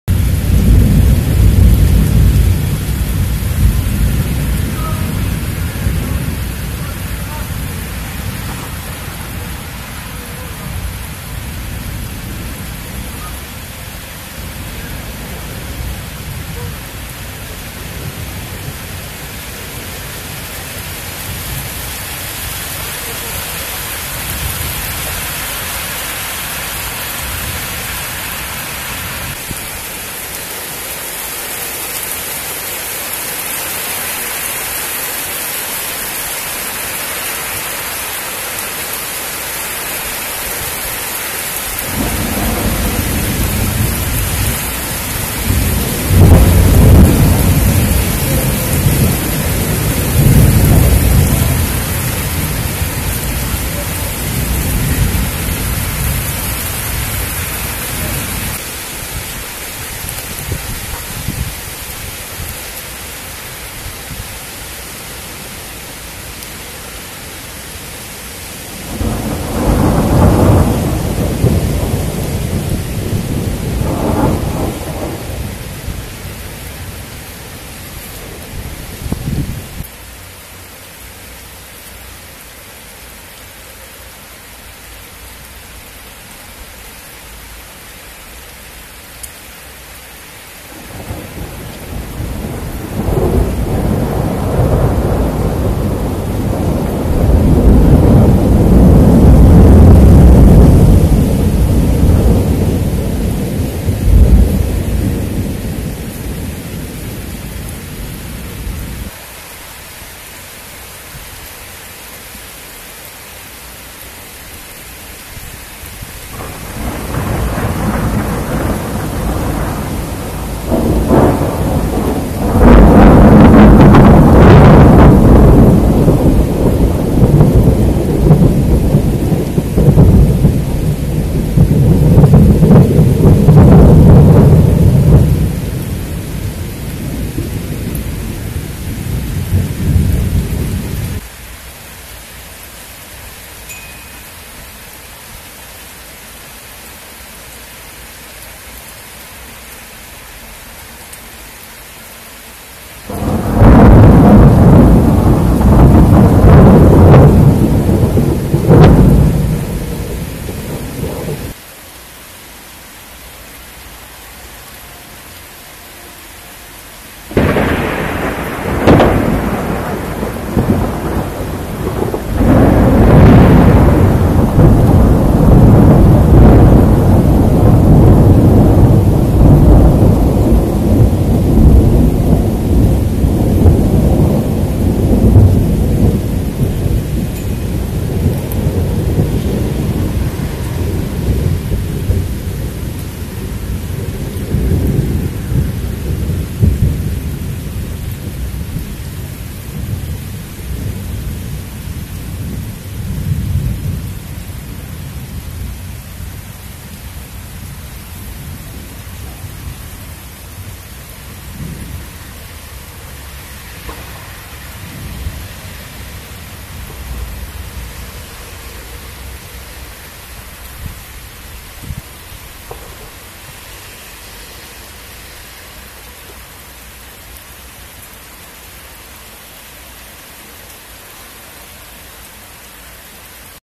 Wyładowania w Poznaniu godzina 23:15 dnia 09.07.2016
Nagranie z którego wyciąłem te strzały trwało 16 minut